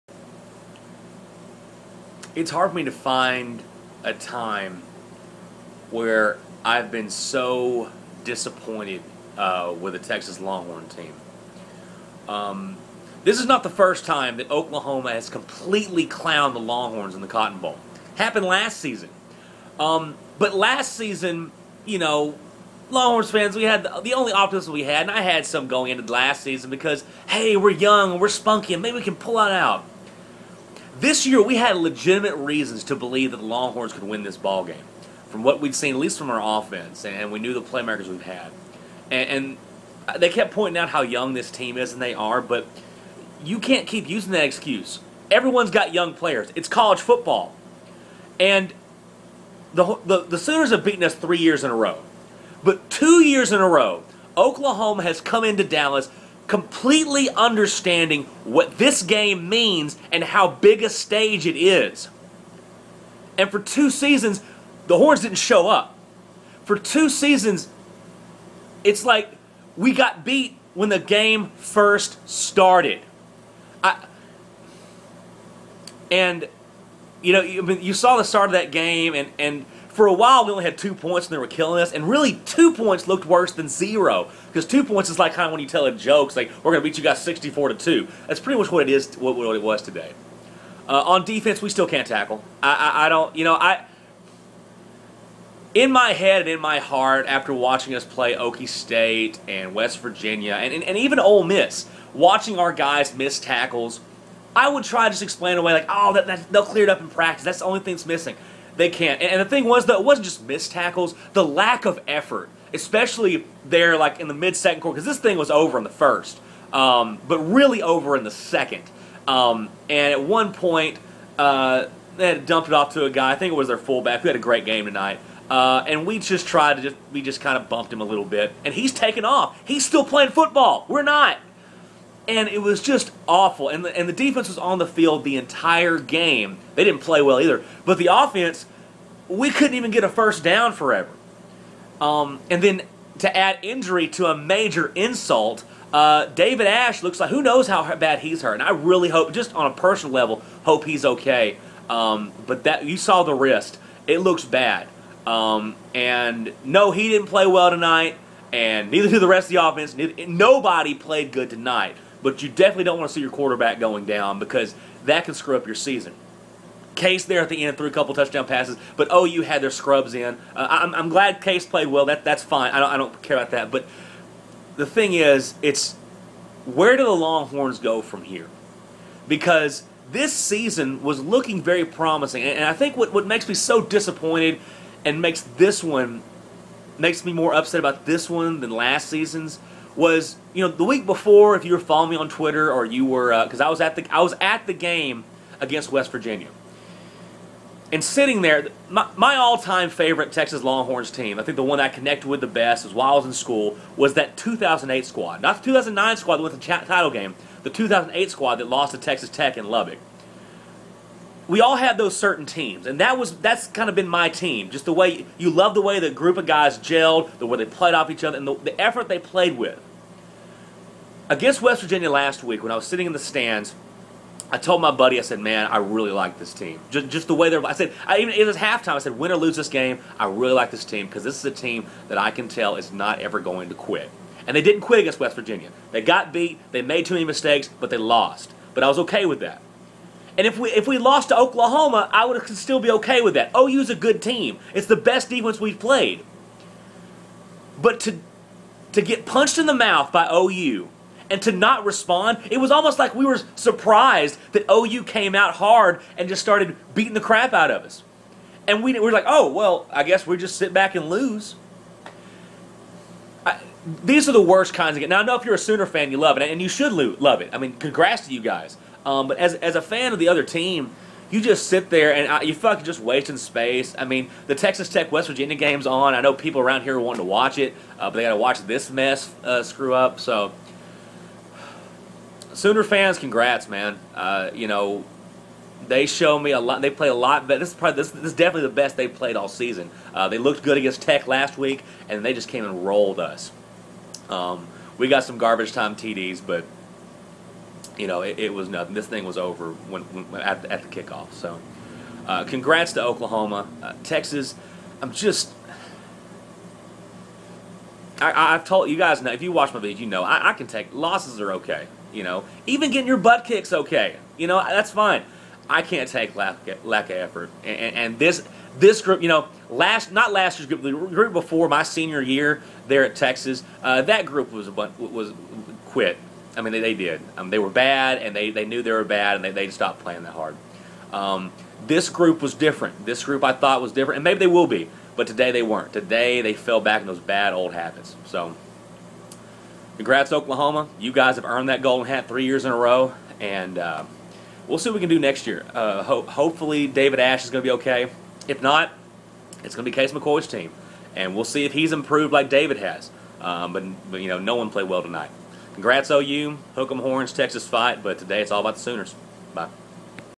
Oklahoma understands this rivalry
The Sooners destroy my Texas Longhorns 63-21, and it wasn't even that close.